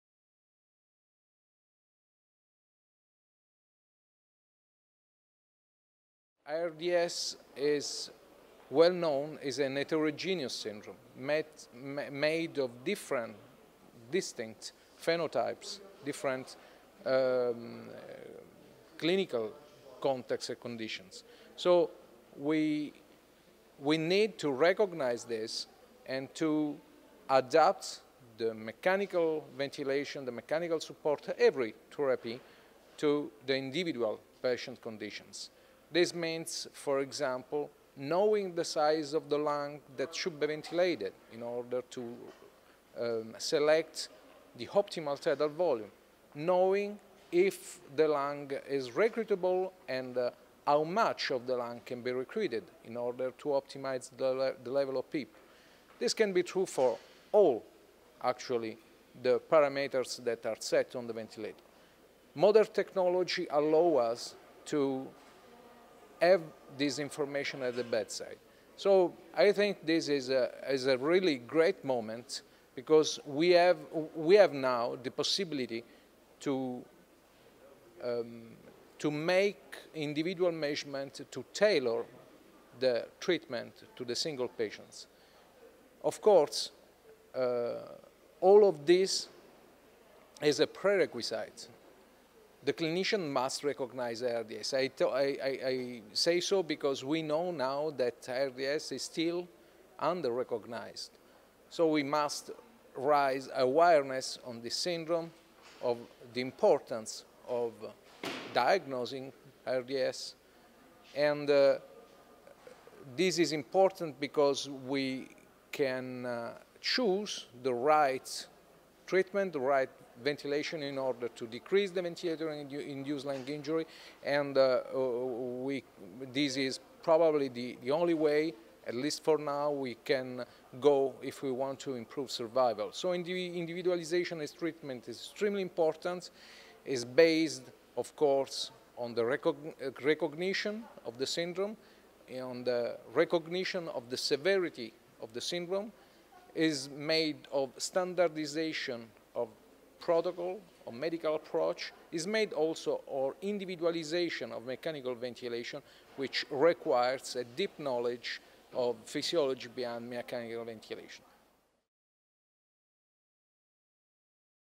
Prof. Salvatore Maggiore @ ESICM LIVES 2016 | GE Healthcare
Prof. Salvatore Maggiore, Speaker of GE symposium on Nutrition and Lung protection, shares the take-away of his lecture on Protective ventilation